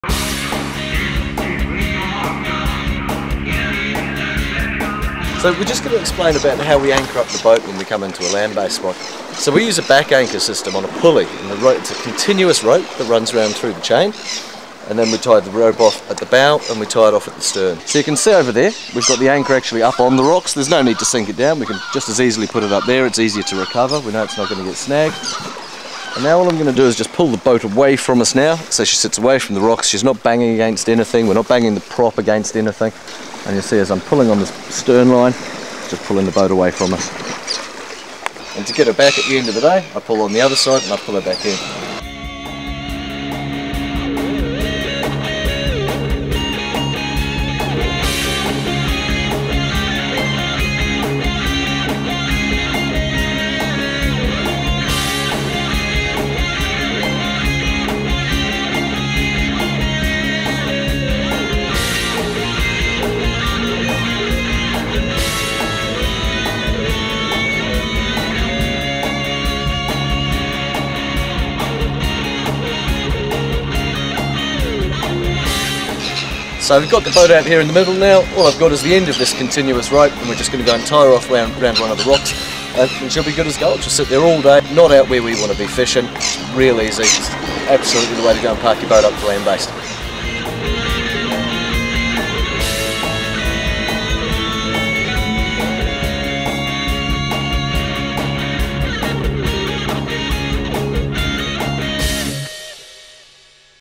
The Back Anchor Sytem
When we're out on some of the mint ledges around NZ, we anchor the baby stabi using the Back Anchor System. This is a continuous floating rope system that allows us to anchor the boat out away from the rocks and is easy to retrieve when we are ready to depart. It's something that impressed us when were shown it and now we're passing on the info. It's best to use floating rope to avoid the rope sinking and snagging :) Tight Lines!